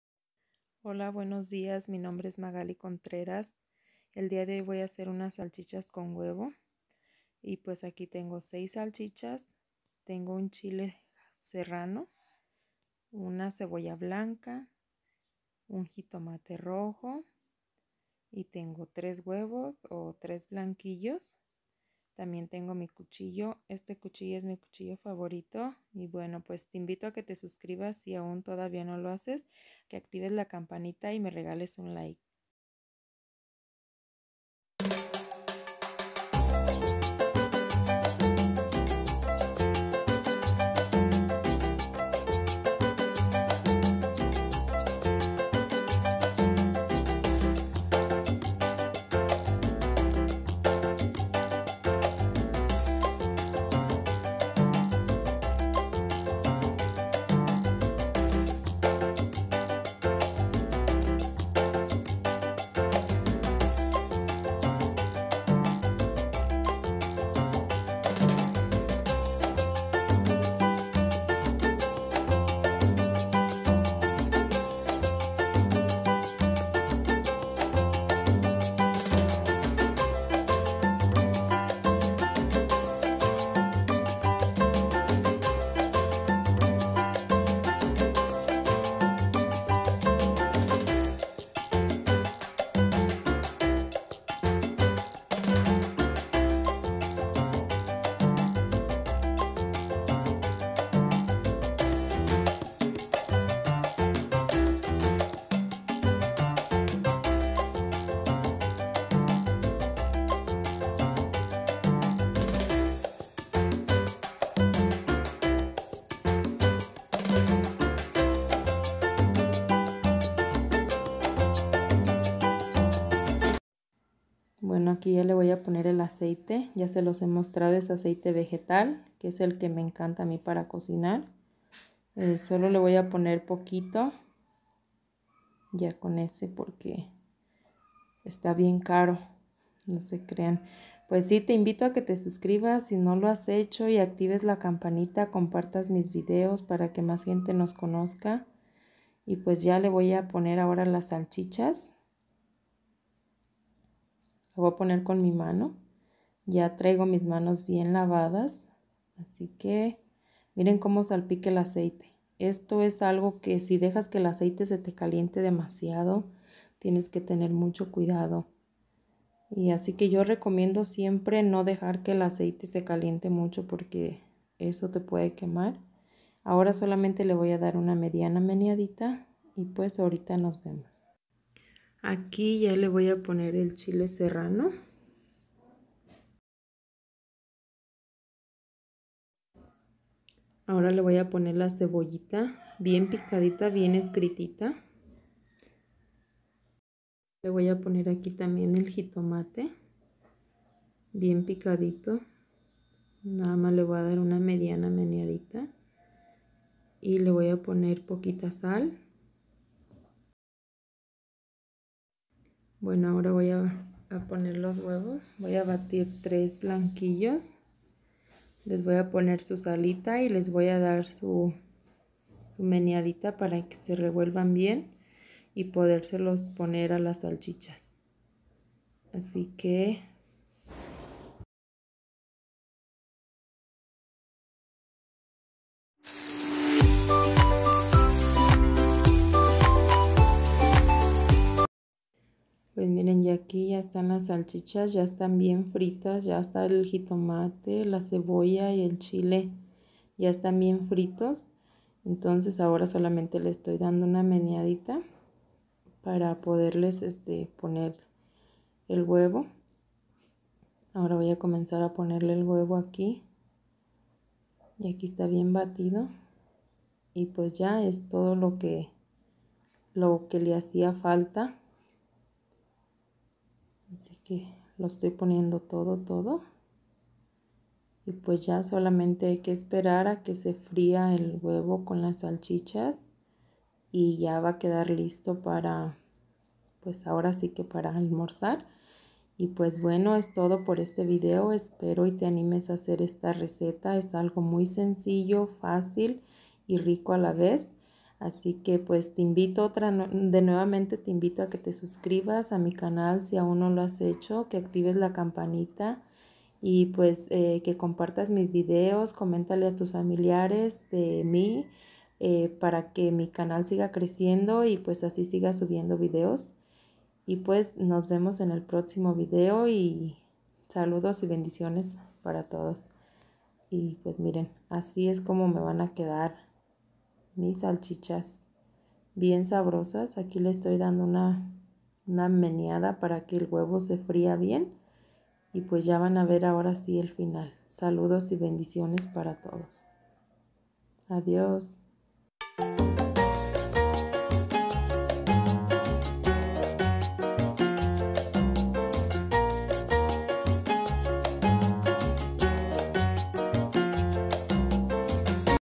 Salchichas ala mexicana#magalicontrerasvlogs❤
Holo bueno pues hoy les comparto una rica y deliciosa reseta de salchichas ala mexicana espero y les guste y seanimen aserla así que si tegusta no olvides darle 👍 y comentar compartir para que mas personas lo miren gracias bendiciones